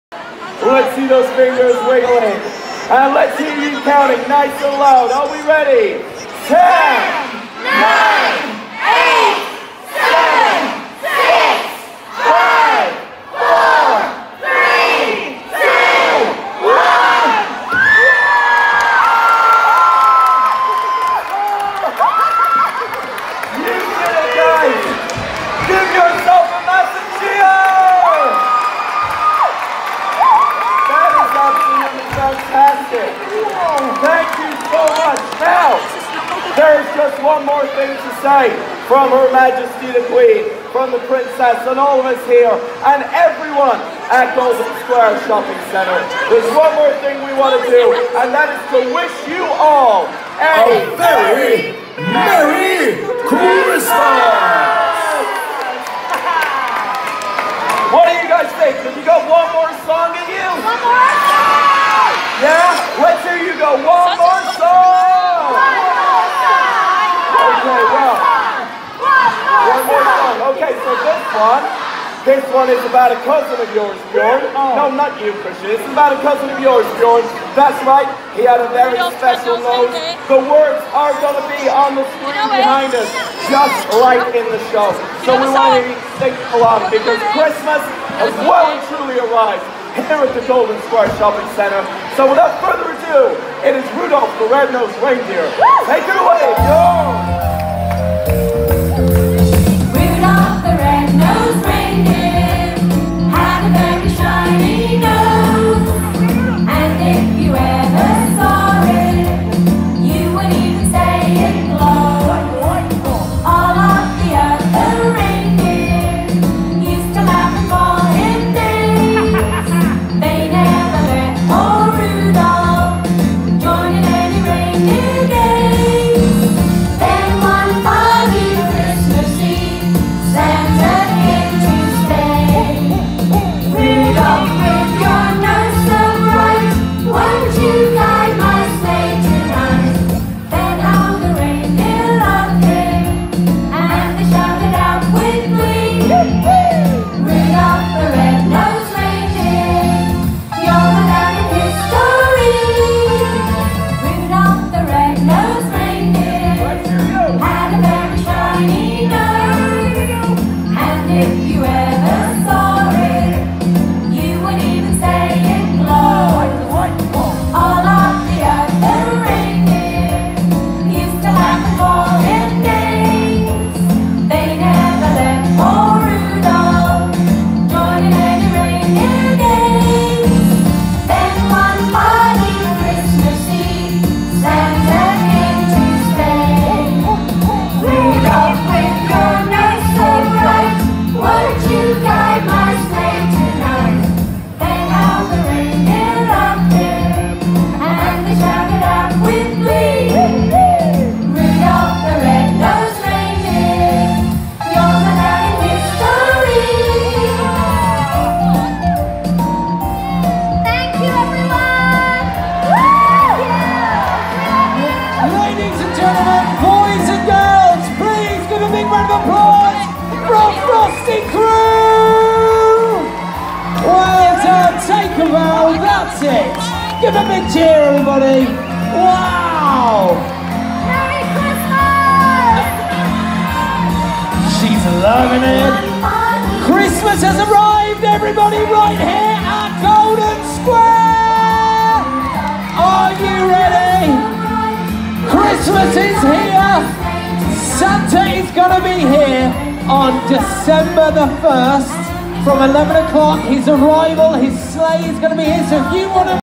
Warrington Christmas Lights Switch on 2018
Short clip of the xmas lights been turned on in my hometown of Warrington 🎅🏼